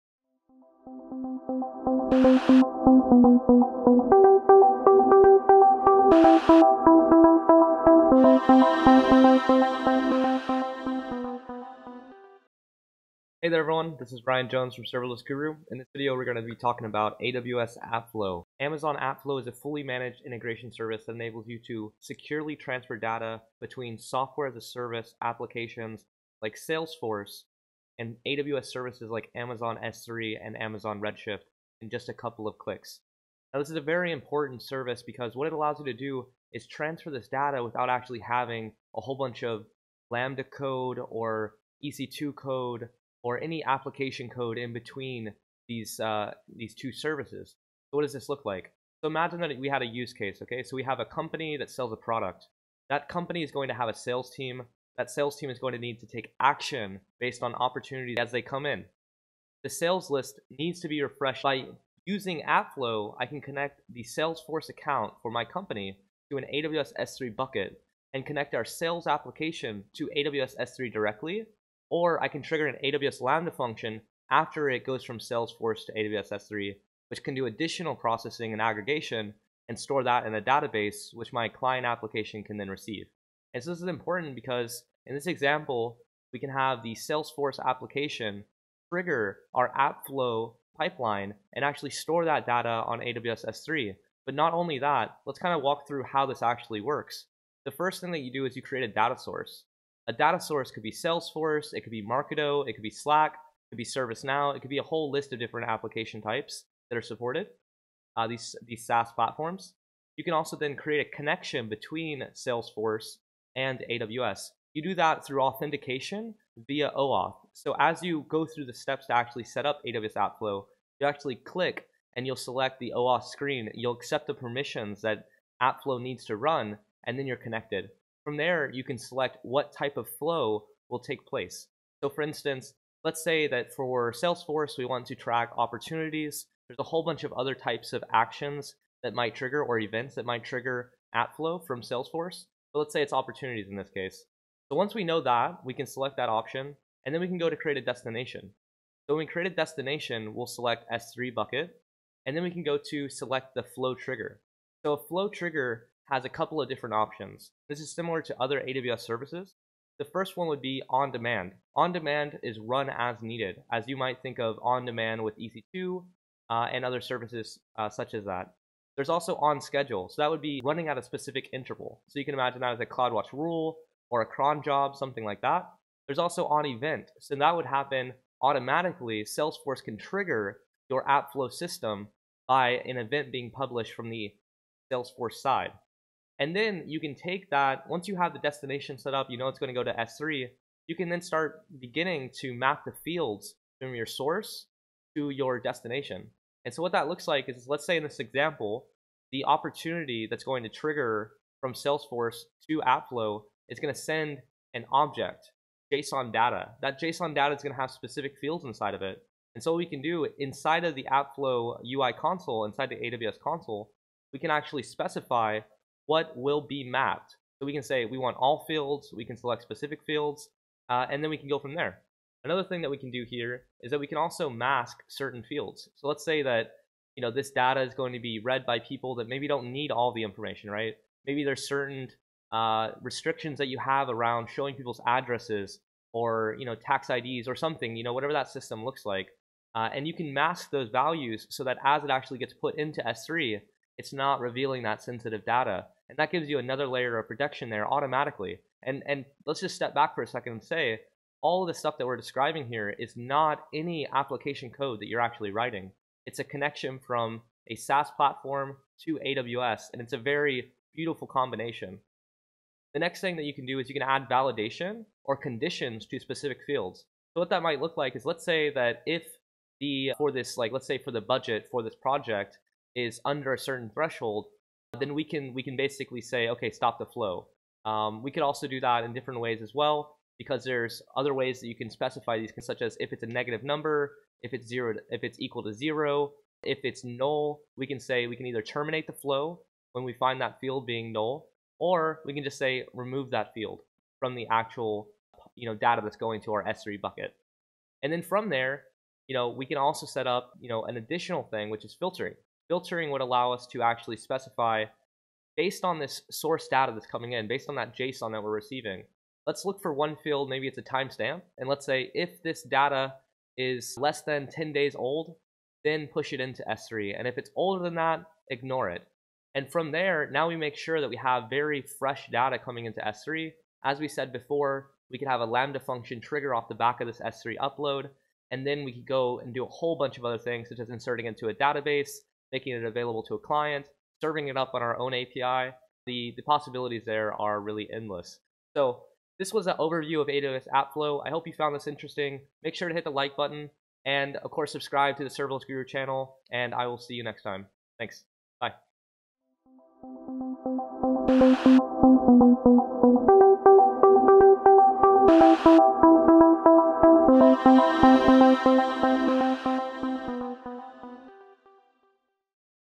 What is AWS AppFlow? How does it work? What does it solve?
AWS AppFlow is a new service by Amazon which allows you to connect SaaS applications directly to AWS services without coding.

In this video, we dive into the service and look at how it works and where it would be useful.